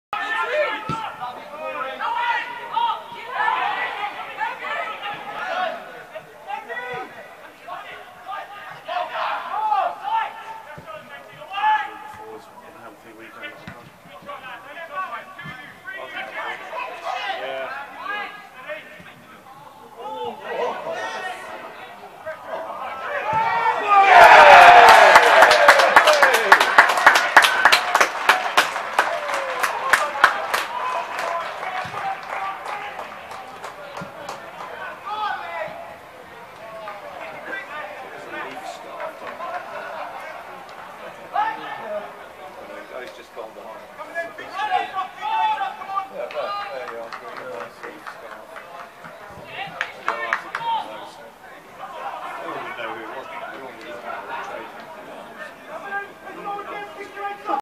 Whyteleafe 4-1 Bowers & Pitsea. Buildbase FA Trophy Extra Preliminary Round. Sat29Sep2018
Missed chances, superb home goalkeeping heroics, then some breakaway goals see Bowers' FA Trophy hopes fall at the first hurdle in leafy Surrey, as Whyteleafe with some clinical finishing at Church Road set up a trip to Hastings in the next round